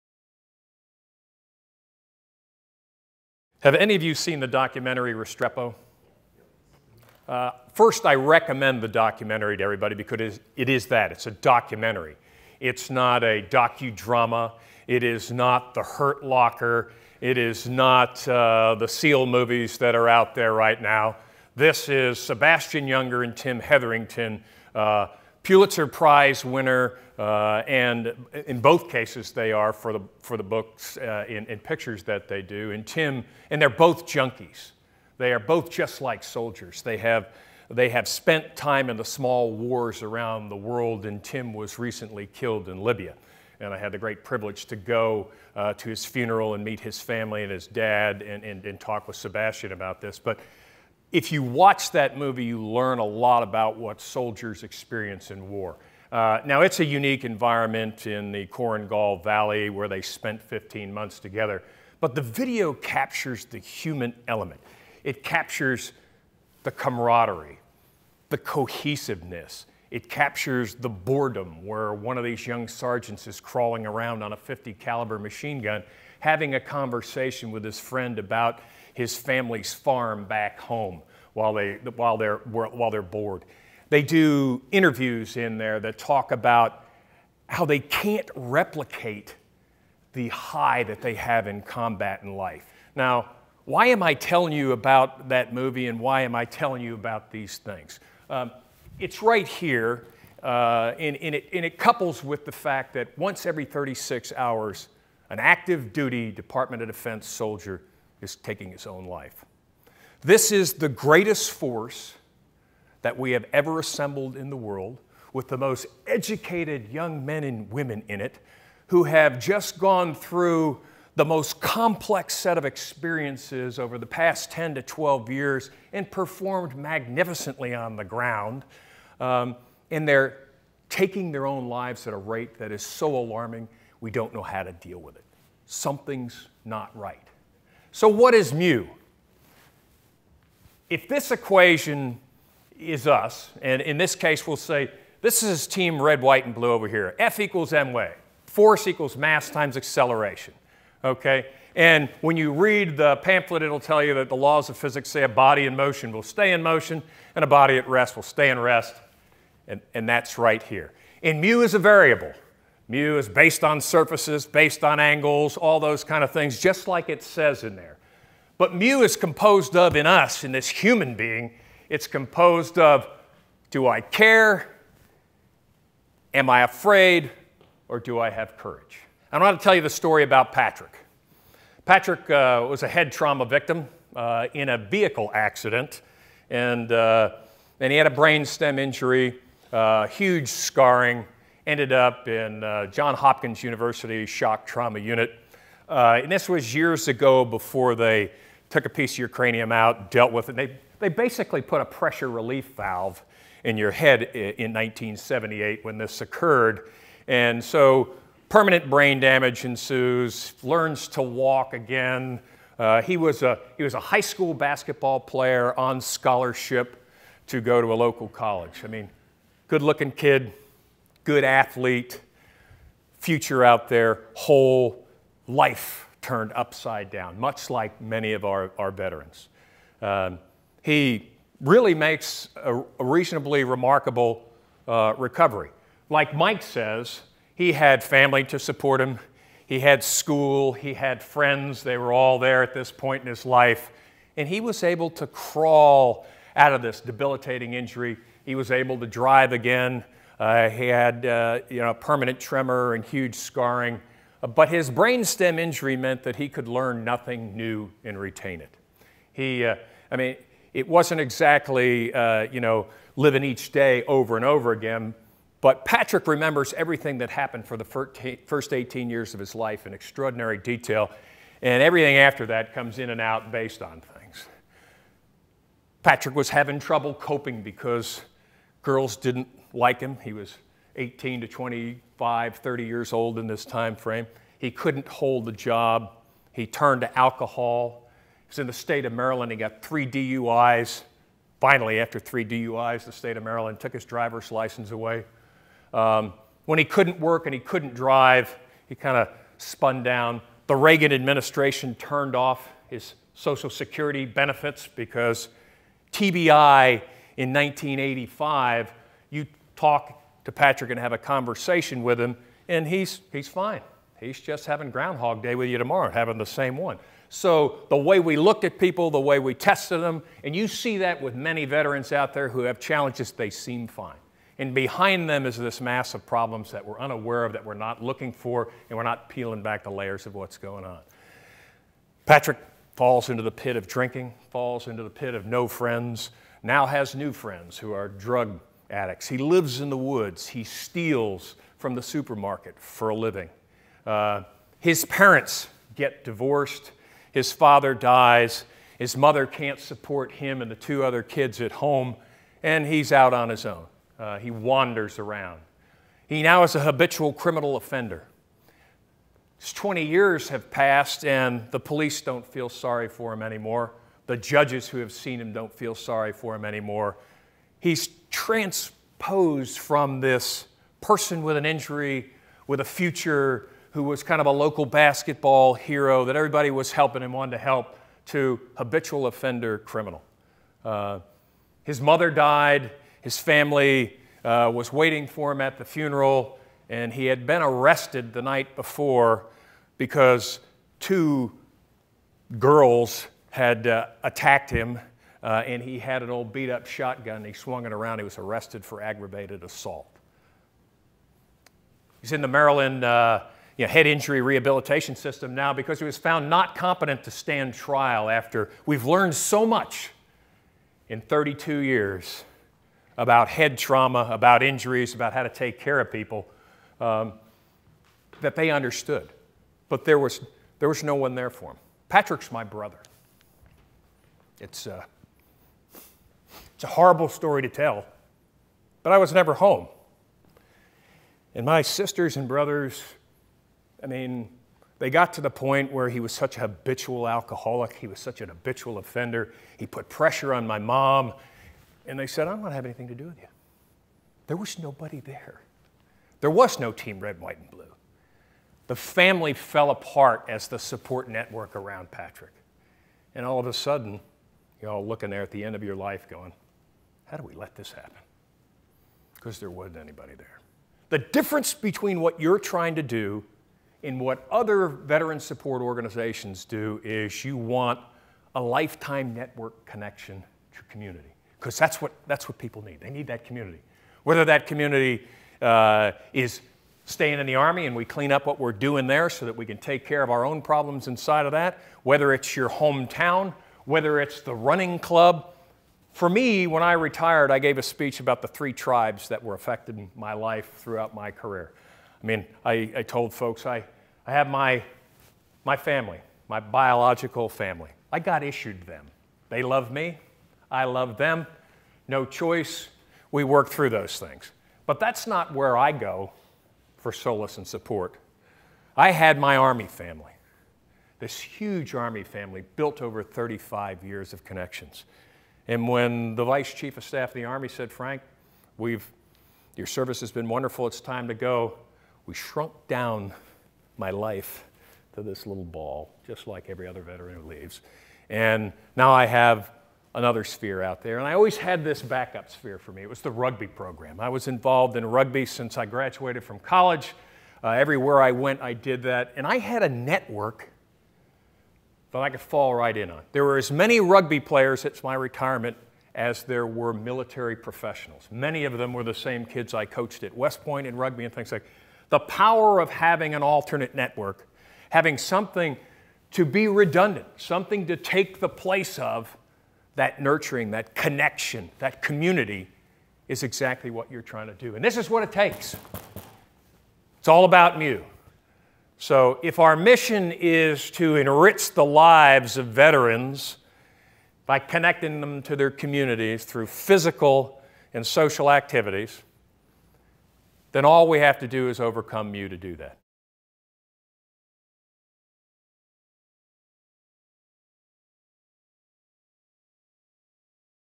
LTG (Retired) Frank Kearney Team RWB 2013 Leadership Summit
LTG (Retired) Frank Kearney, Director of the Team RWB Advisory Board, presented at the Team Red, White & Blue Leadership Summit in January of 2013. Kearney's presentation talks about veteran suicide rates and the motivation behind the mission of Team RWB. Through personal stories and a very driven commitment he shows the importance of creating a strong community .

The summit was held for two days in the Pritzker Military Library bringing together RWB Leadership from across the country in downtown Chicago.